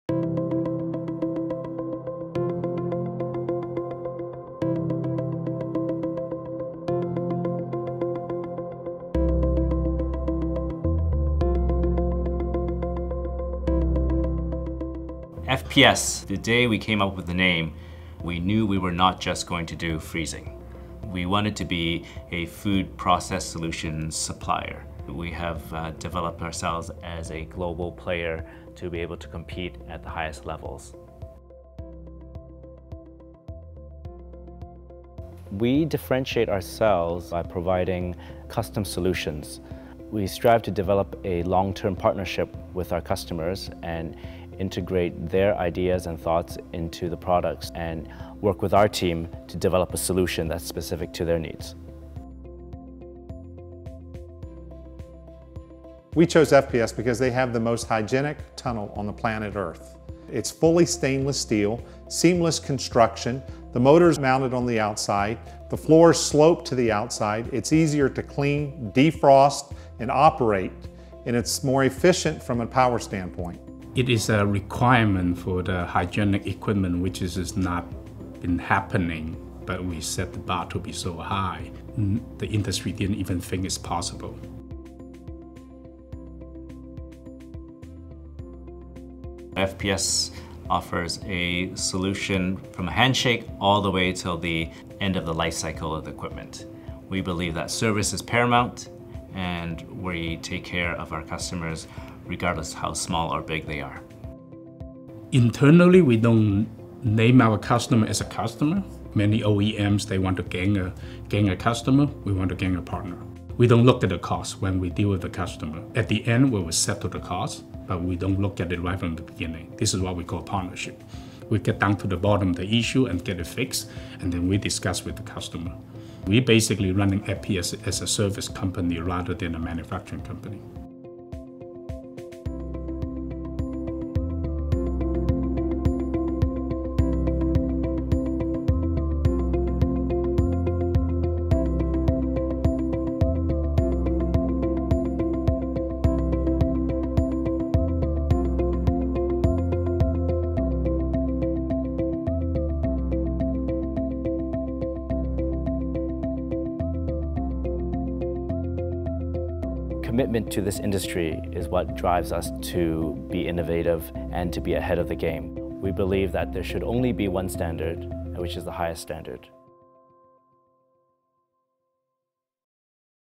FPS Food Process Solutions Corporate Message 2023
FPS Food Process Solutions is a global leader in turn-key food freezing and cooling equipment. We provide innovative systems to ensure the highest efficiencies and to meet the most stringent sanitary demands. Our corporate 2023 message demonstrates our global expansion to meet customer needs in food processing solutions.